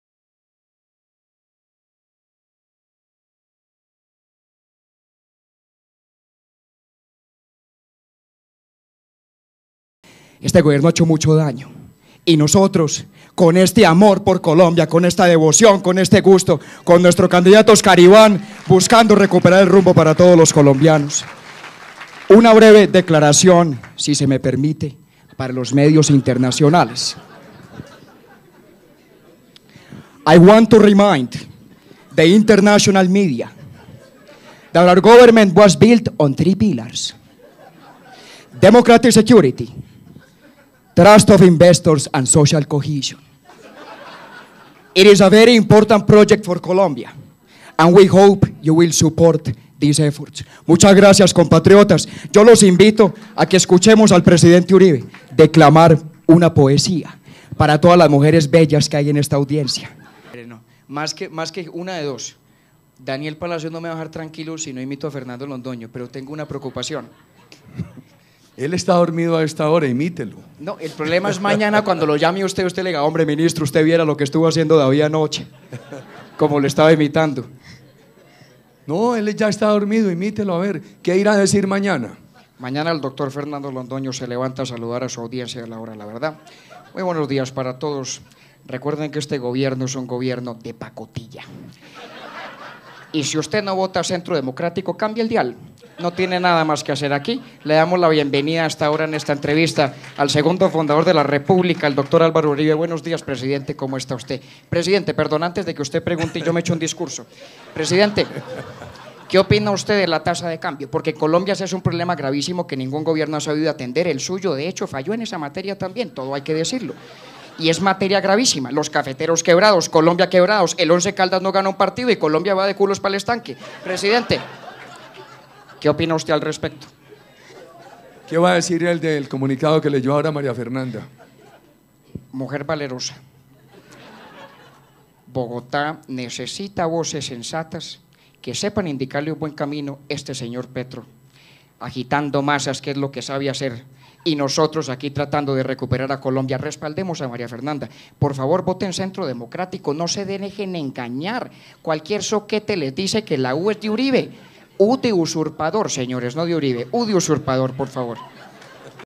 El hijo de Óscar Iván Zuluaga imitando a Uribe y a Fernando Londoño
Evento del Partido Centro Democrático llevado acabo en el Hotel Dann Carlton de Bogotá.